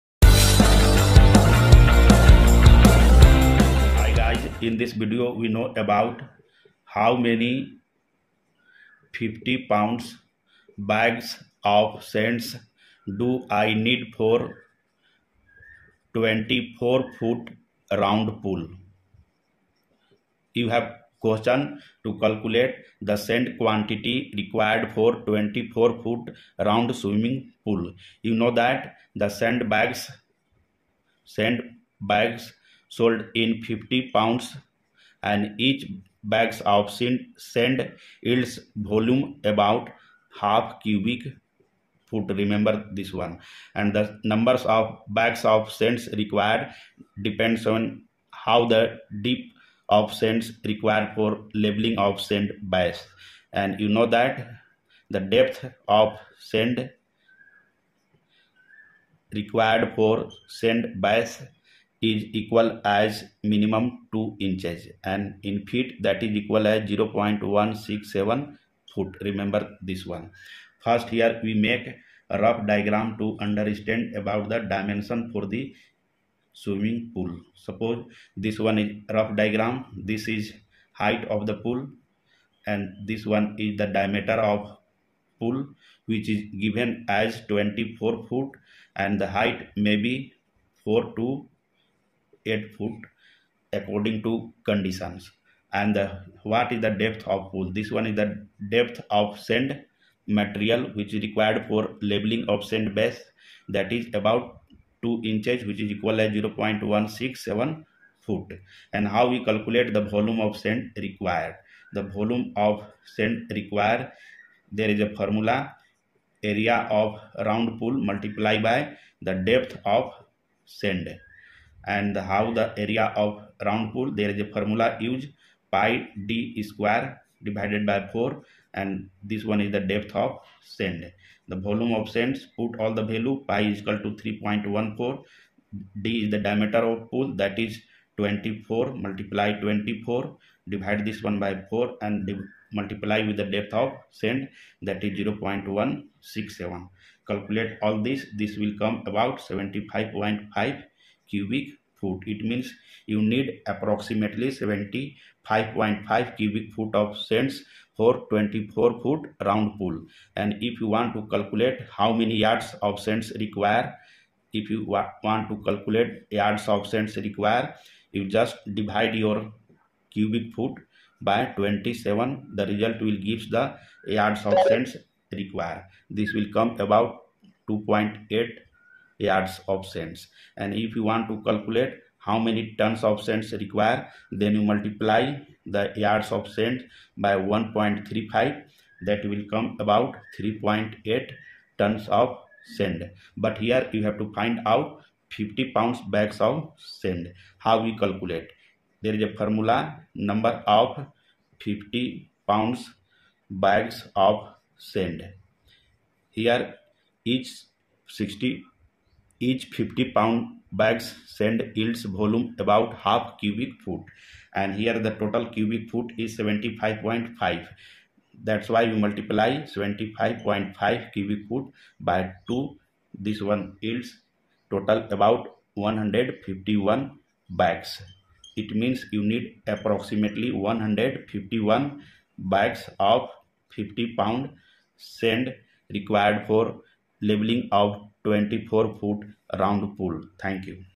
How many 50 lb bags of sand for 24 foot pool | Yards, cubic feet, or tons of sand for 24' pool
@HelloSirJi How many 50 lb bags of sand for 24 foot pool | Yards, cubic feet, or tons of sand for 24' pool

To determine how many 50 lb bags of sand you need for a 24-foot pool, you'll need to calculate the volume of sand required to create a stable base. This calculation depends on the depth of the sand layer you want to lay.

Typically for a 24 foot round pool, you will need approximately 75.5 cubic feet, 2.8 cubic yards, 3.8 tons, or 151 x 50 lb bags of sand for at least 2 inches of sand depth.